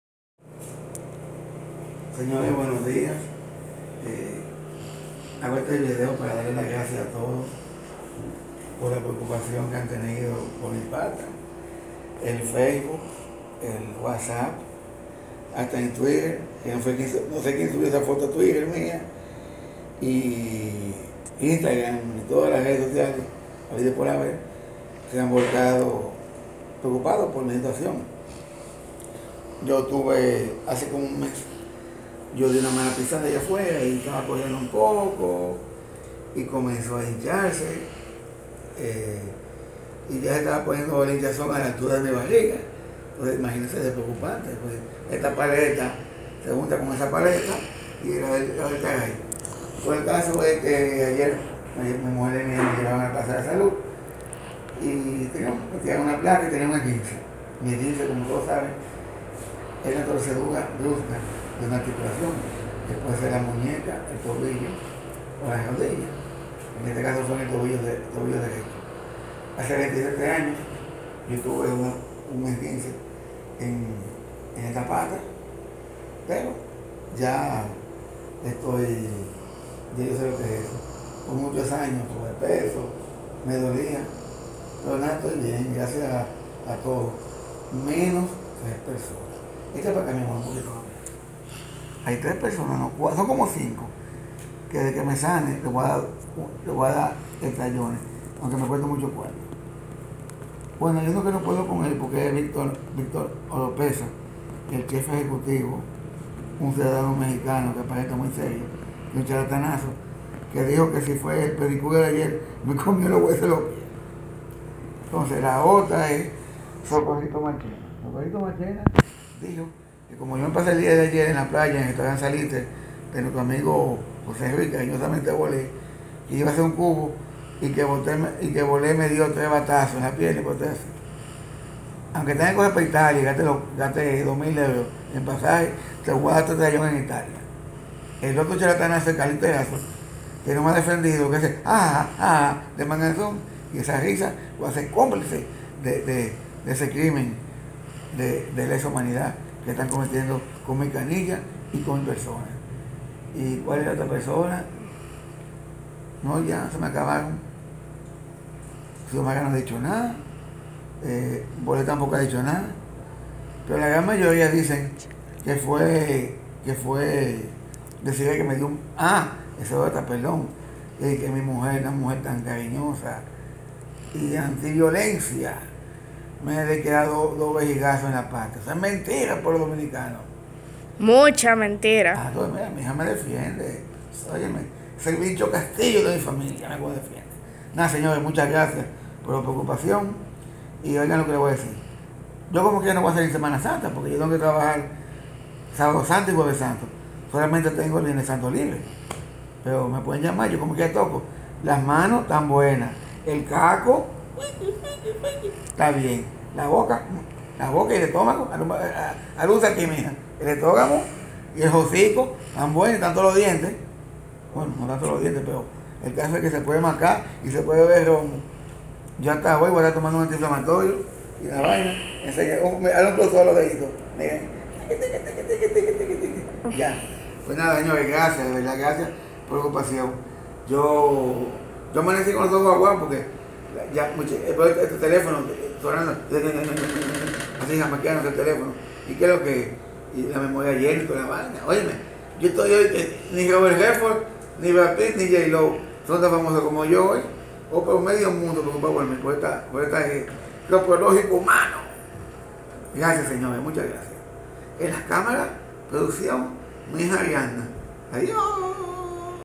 Explicación de mi problema canillística actual.
Esguince en el tobillo derecho.